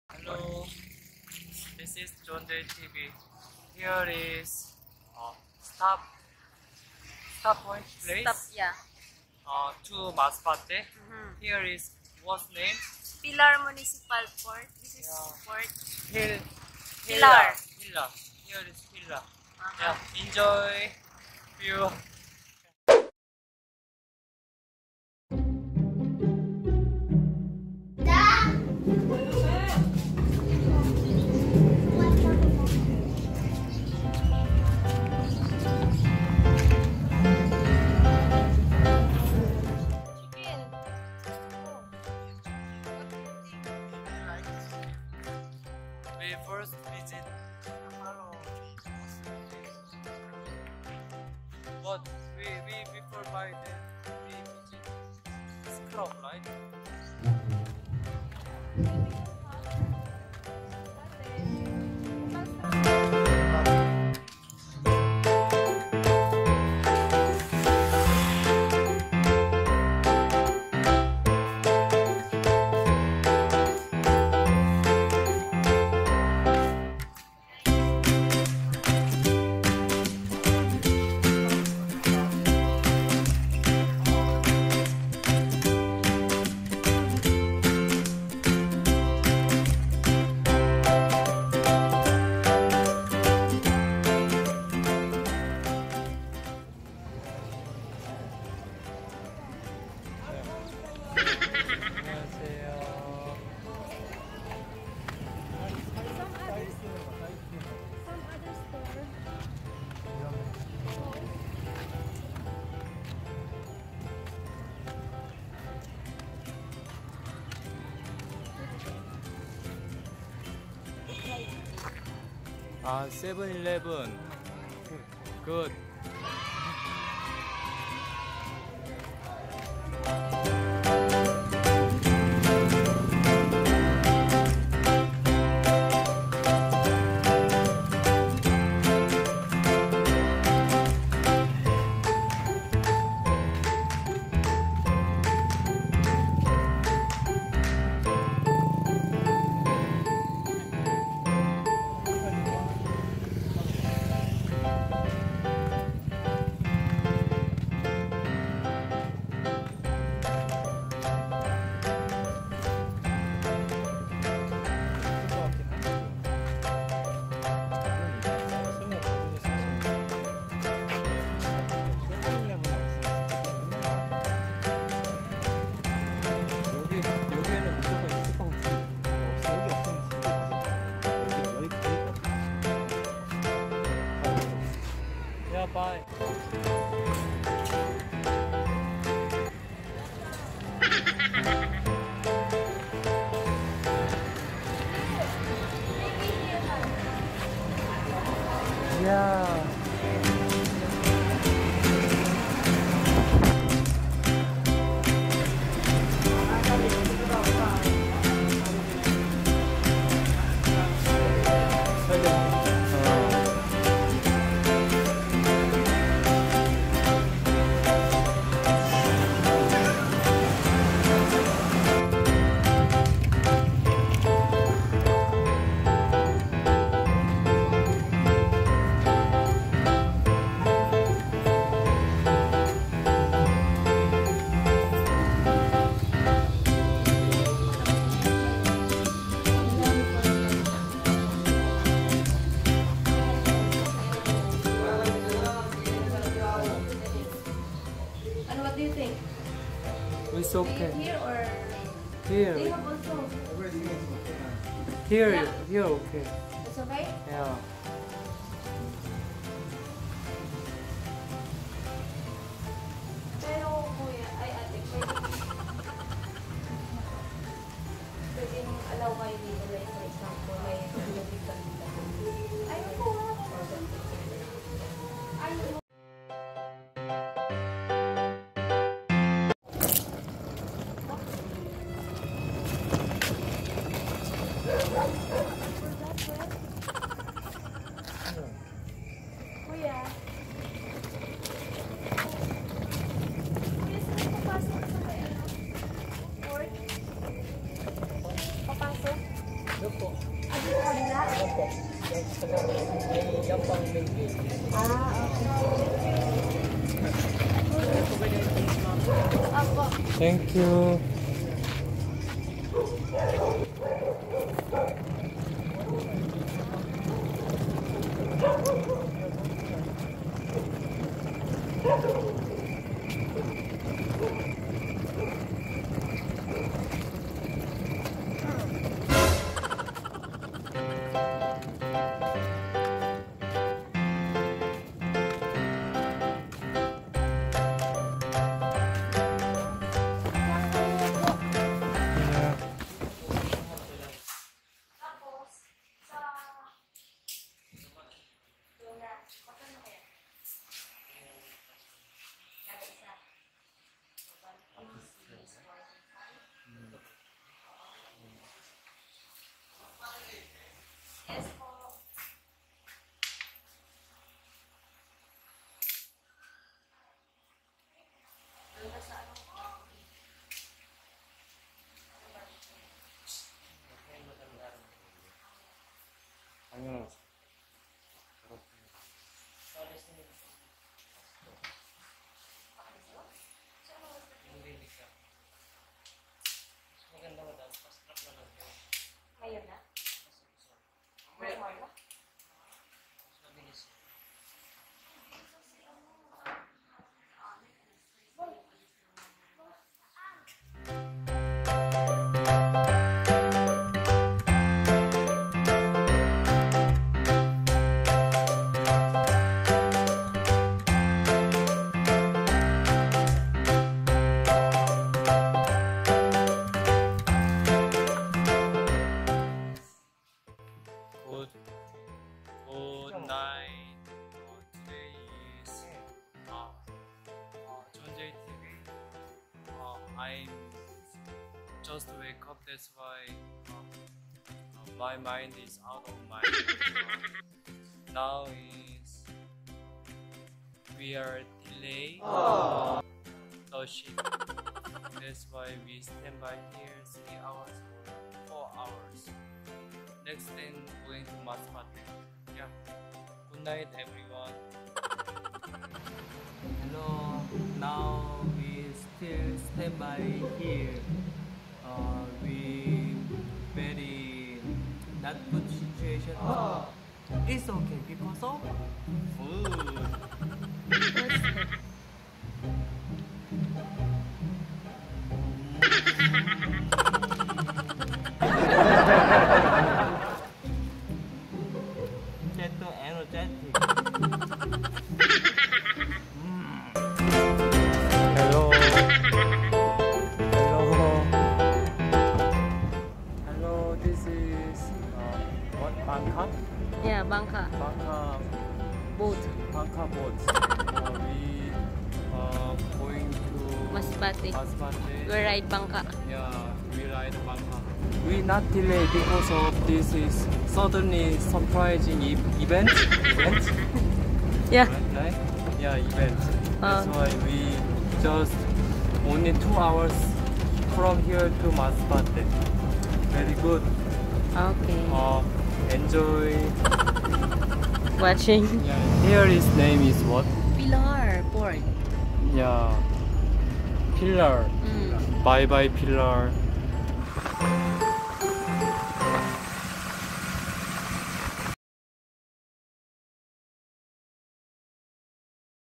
what's in Pilar Philippines (stopover @Pilar port) 2019 tour in the Philippines
Stop over at Pilar port going to Masbate city
walking around at pilar public market
with korinoy friends.